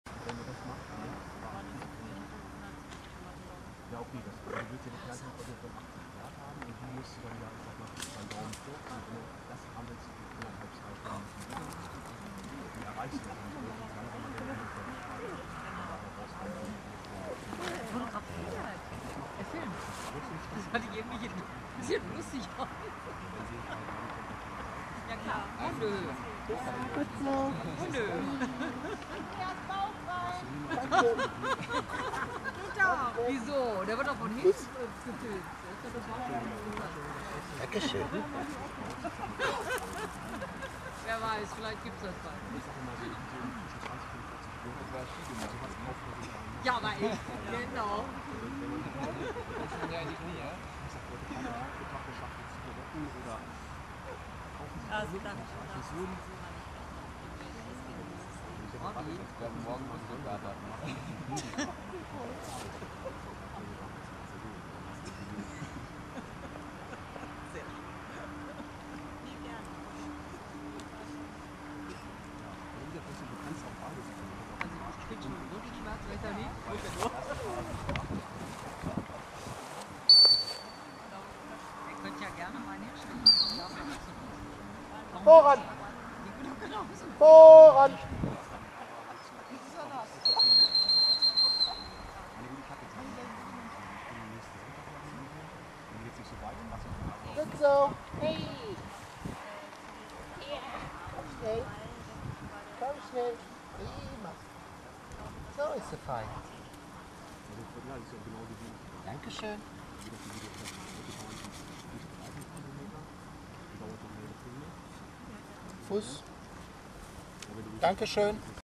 Dummy Training Doppelmarkierung mit Maja
Dummy Training mit Herbert, am 1.Advent 2013 in der Sandkuhle Wunstorf.
Doppelmarkierung für Andreas mit Maja (Flat Coated Retriever)